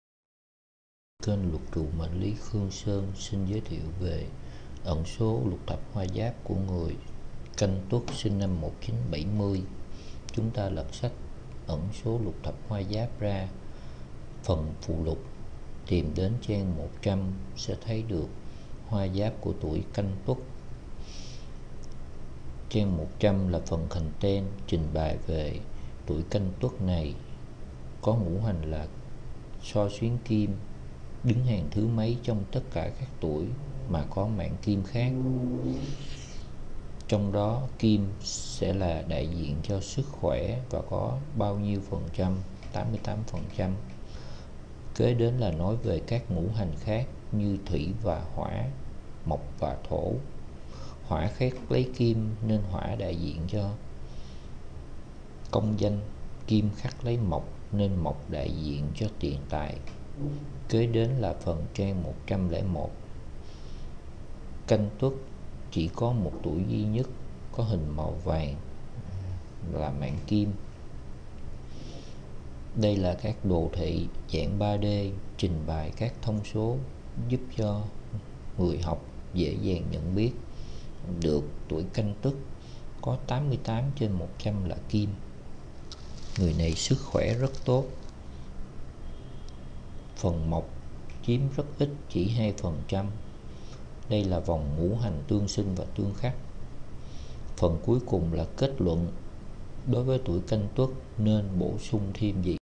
Canh Tuất 1970 xoa xuyến kim ( Lục Thập Hoa Giáp )
Canh Tuất là hoa giáp có kim rất nhiều chiêm 88/100
Vì canh thiên can chứa Kim
Tuất địa chi cũng chứ kim